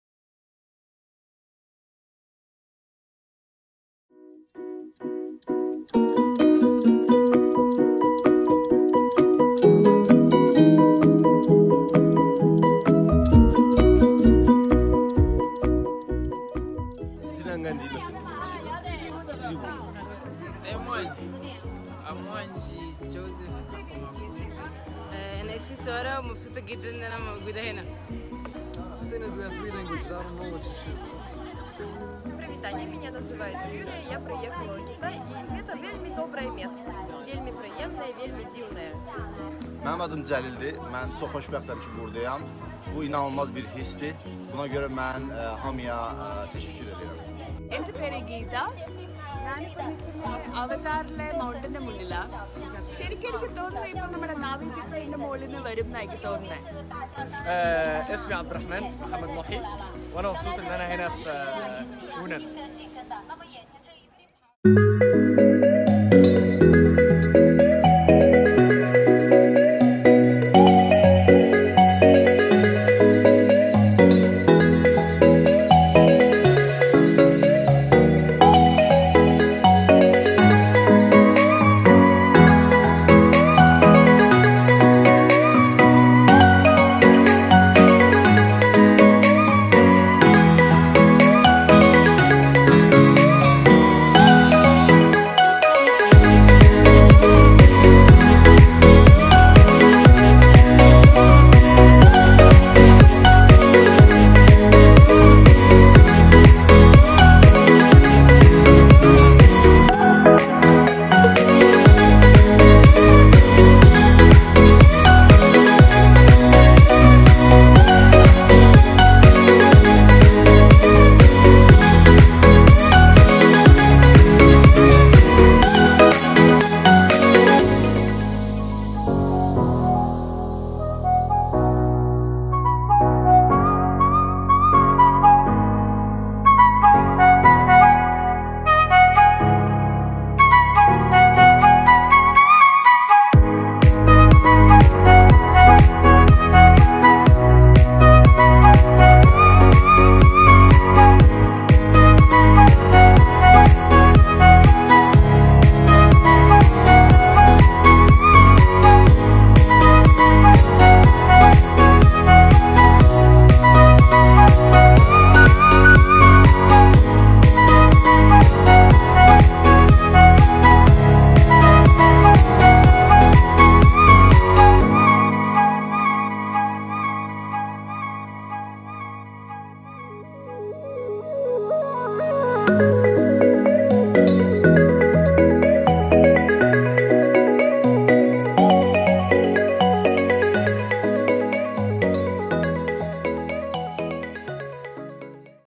United Nations Young Artists Residency Program promotion video
The Changsha Municipal Government entrusted UNESCO to select and represent 8 young artists, including cultural and art media, documentary, economic and art management, new media art, music and art, and creative arts and cultural heritage protection. The project lasts for 3 weeks. Young artists will spend half a month exploring Changsha’s natural, historical, human resources, and cultural and media arts projects. In the last week, the artist will combine their experience with the inspiration of the actual study to create the work. After that, the work will be exhibited.